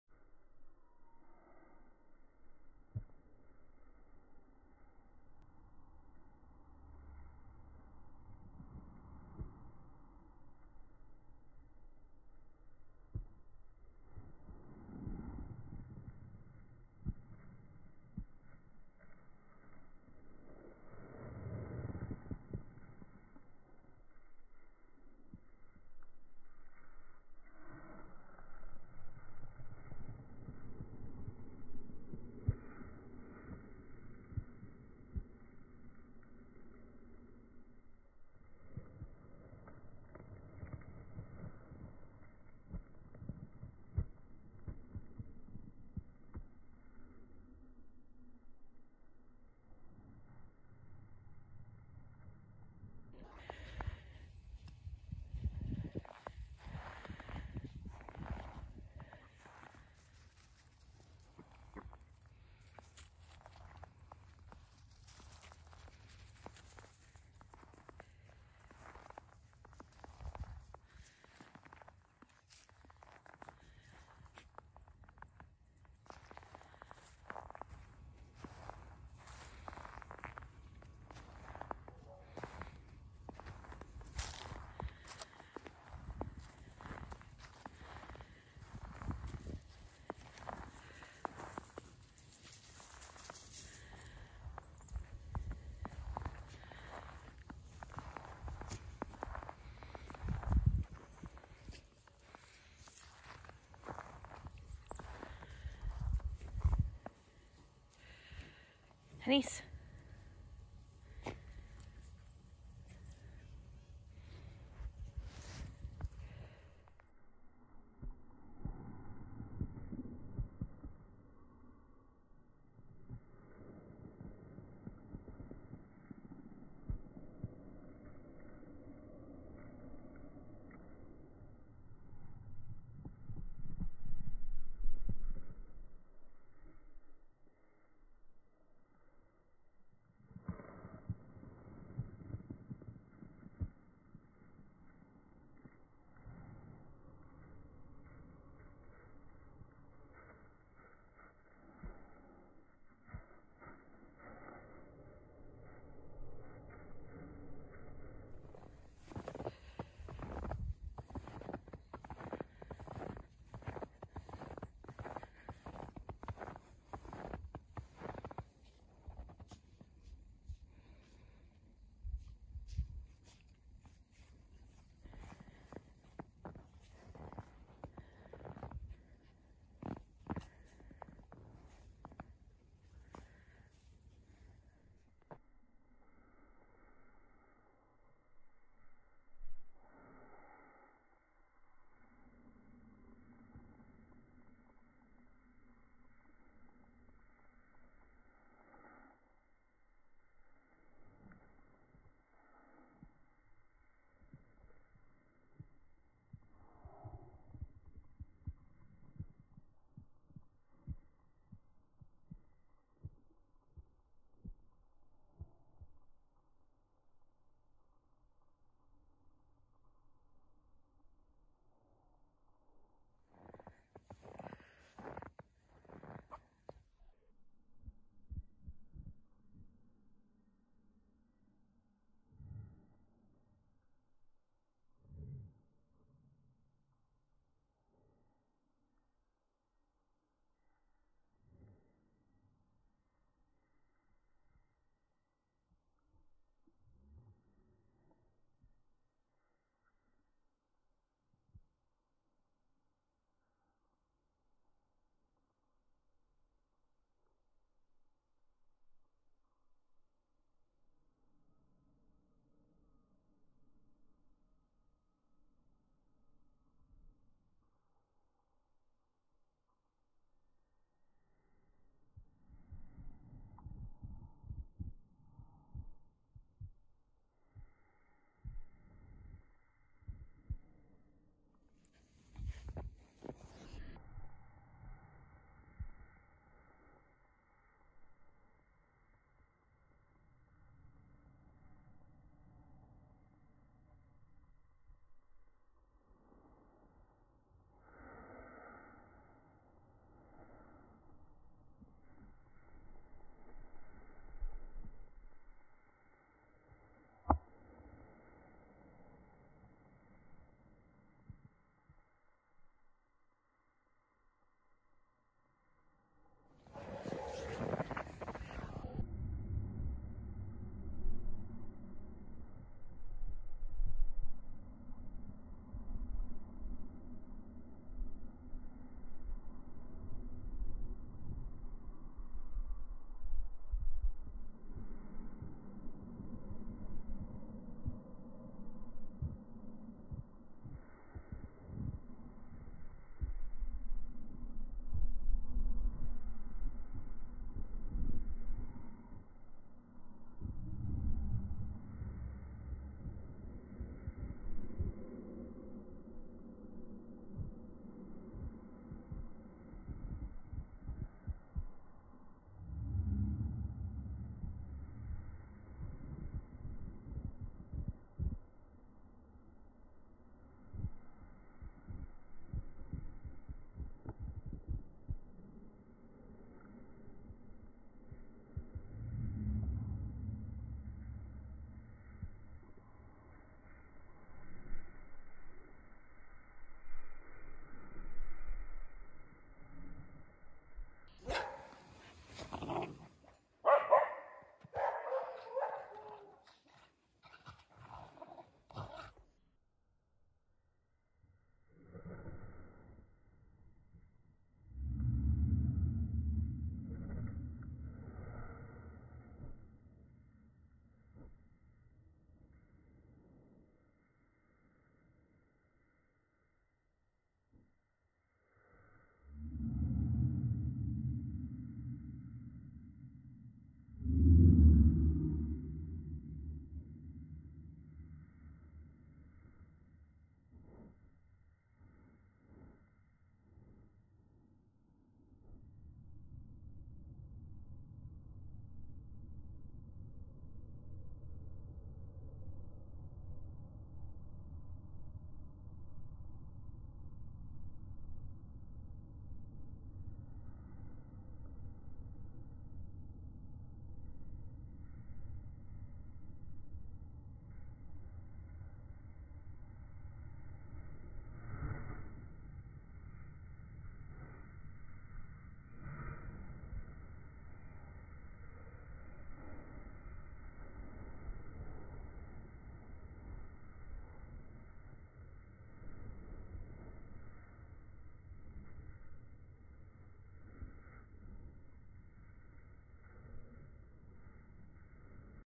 The Polar Vortex 2021 Poodle Style
Poodles enjoying the snow!!!